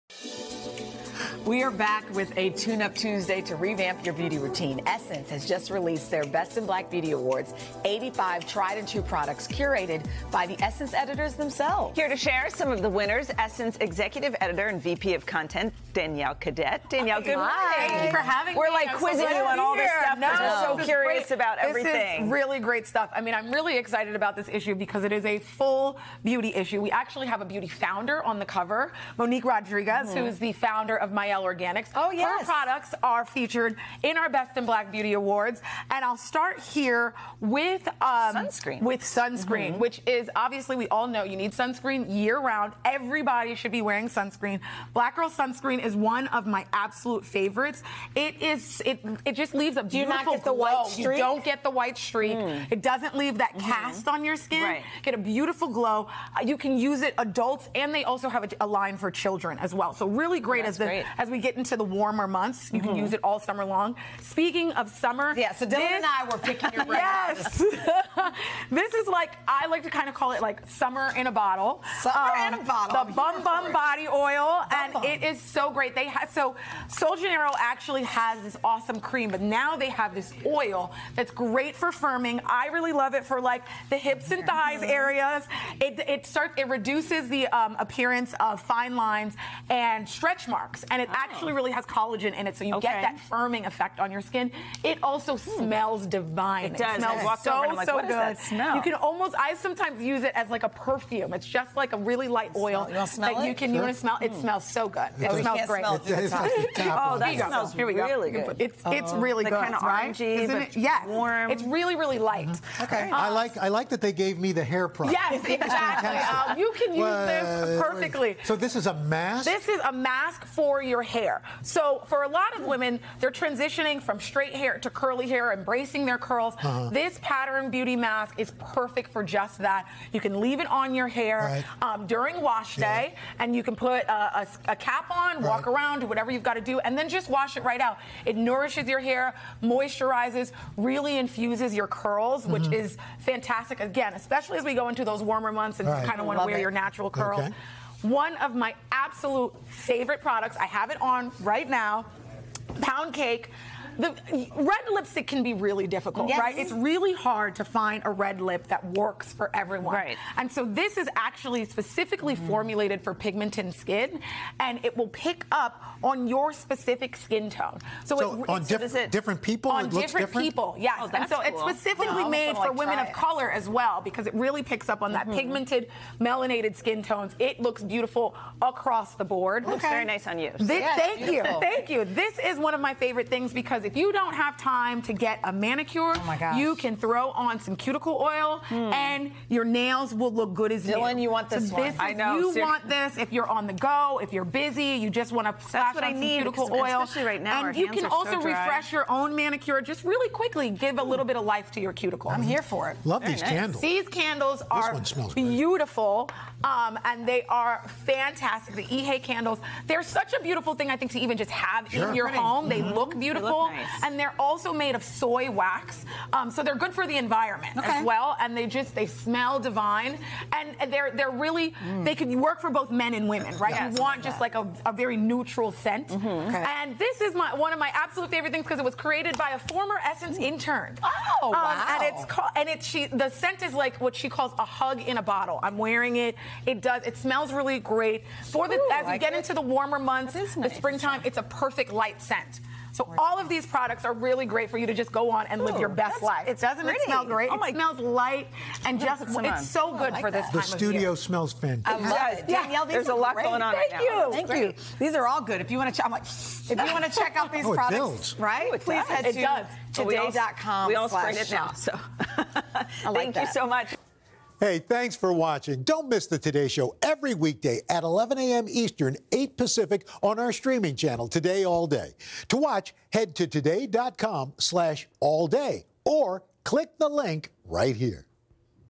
Shop these award-winning favorites in makeup, skin care, more
Essence magazine executive director and VP of content, Danielle Cadet shares the magazine’s “Best in Black Beauty” award winning products to add to your beauty routine for your skin, hair and nails.